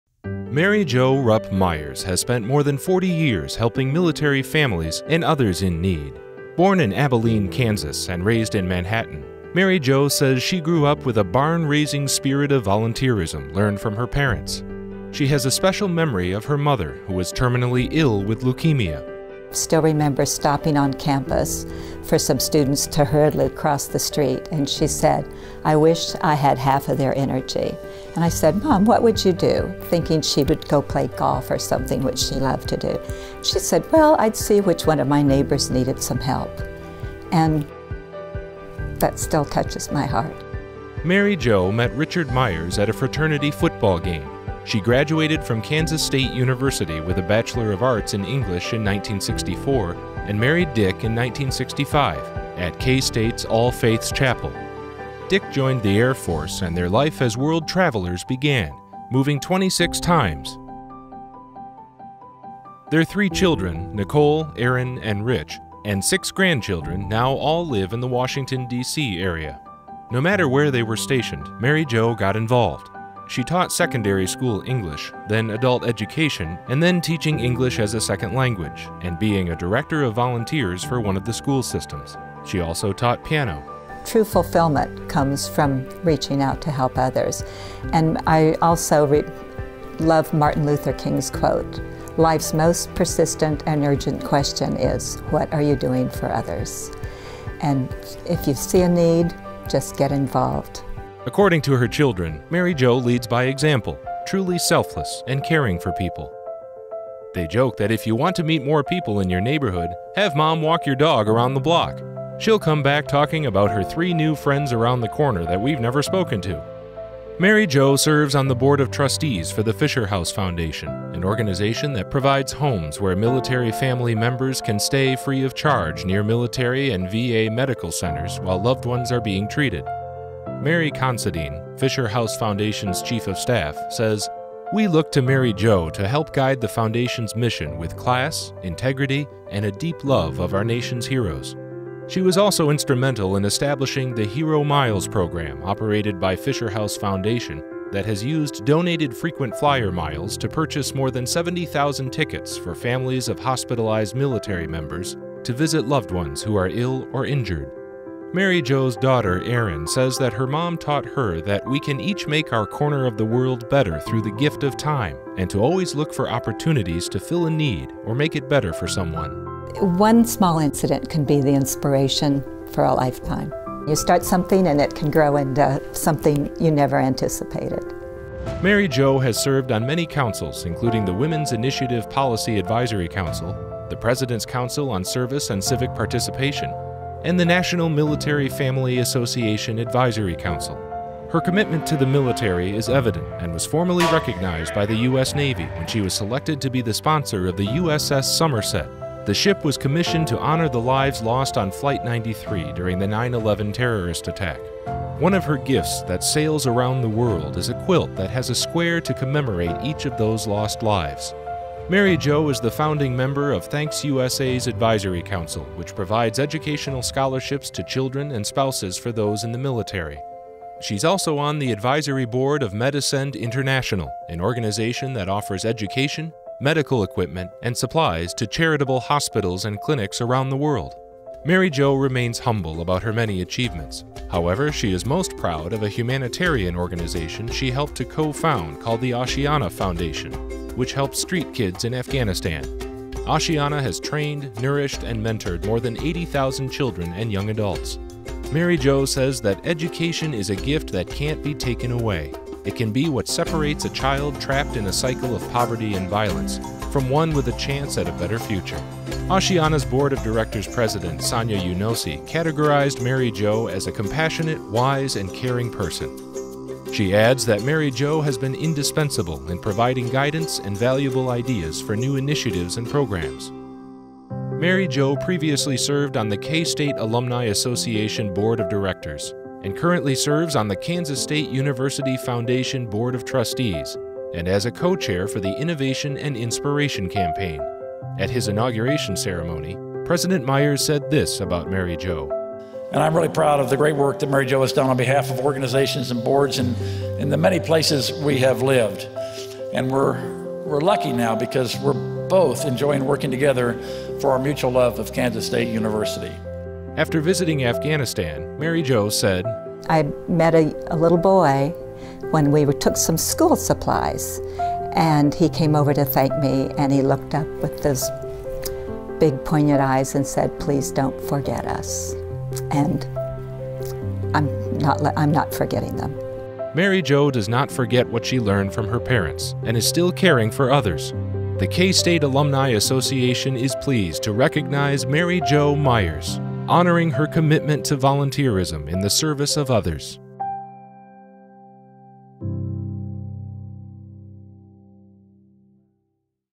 Mary Jo Myers | Alumni Excellence Award
2018 Recipient
Mary Jo Rupp Myers '64

Mary Jo Rupp Myers has a rich background in public service related to military, educational and humanitarian organizations and programs.